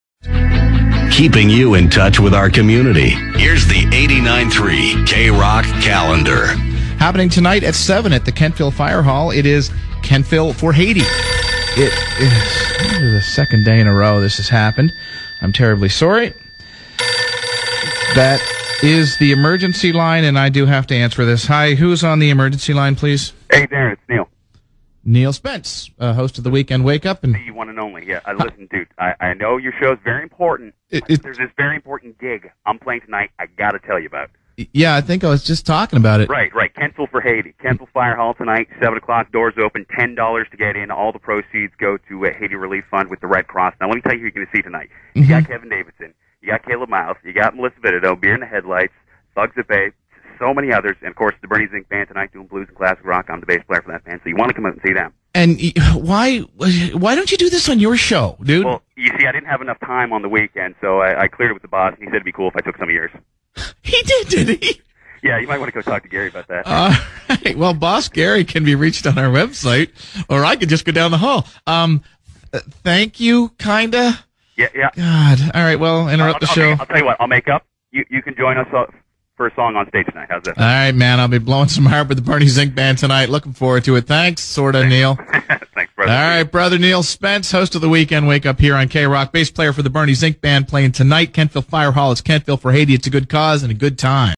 Neil Spence Interrupts Darrin Harvey on 89.3 K-Rock
I busted onto Uncle Darrin's show (because I have that kind of power) to talk about Kentville for Haiti. A benefit at the Kentville Fire Hall, Fri Feb 19, 7pm, $10 at the door. I'm playing with The Bernie Zinck Band.